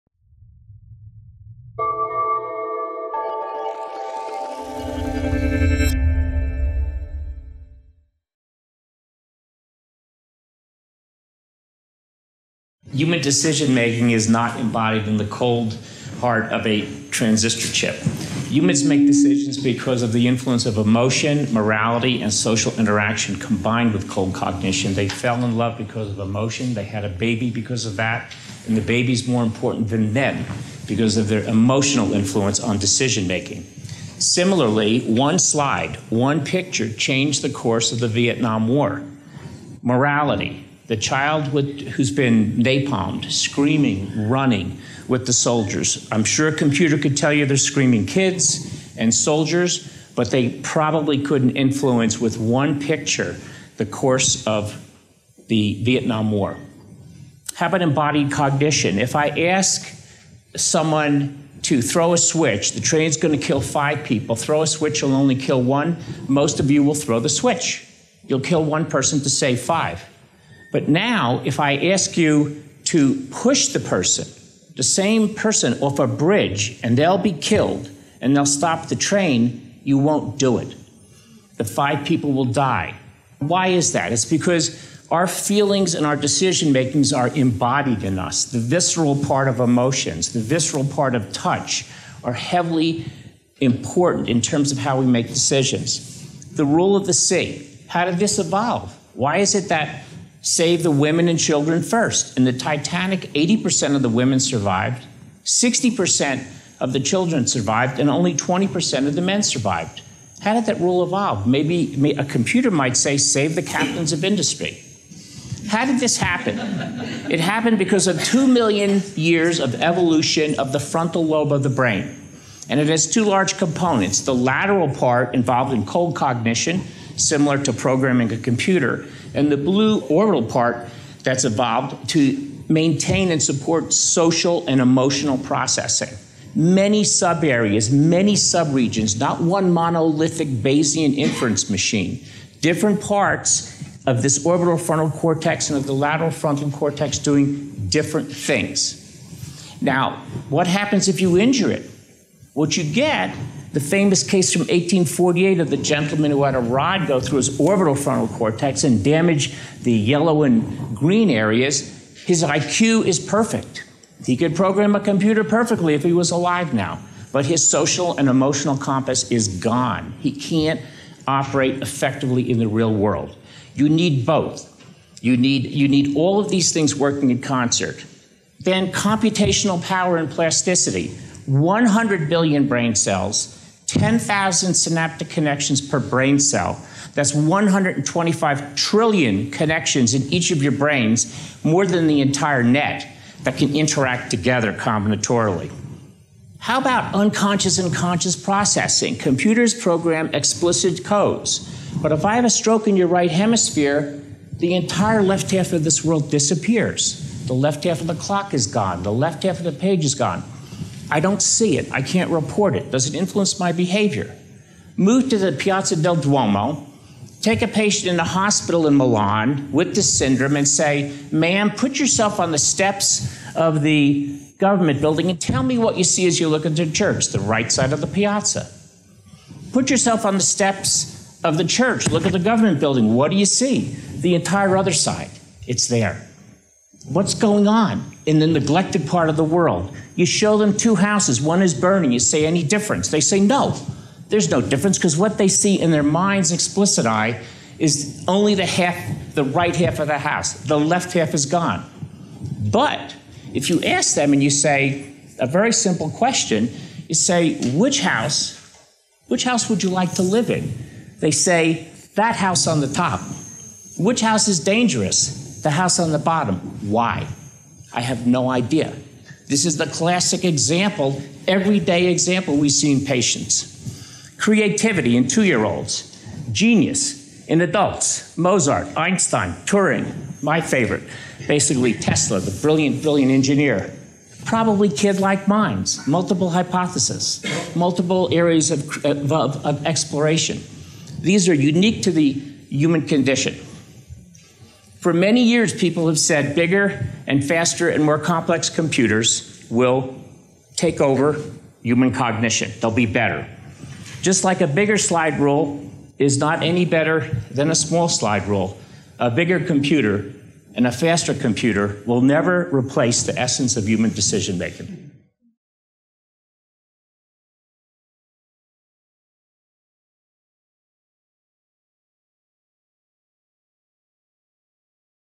The Complexity of Human Decision Making | Robert Thomas Knight
Neuroscientist Bob Knight from the University of California, Berkeley, says our emotions influence the decisions we make, and computers will never be able to match the complexities of human decision making.